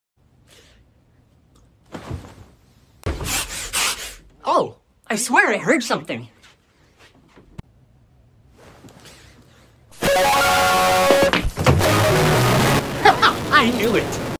Getting Caught On His Phone When He’s Supposed To Be Sleeping
Heres a skit of a kid getting caught on his phone when he’s supposed to be sleeping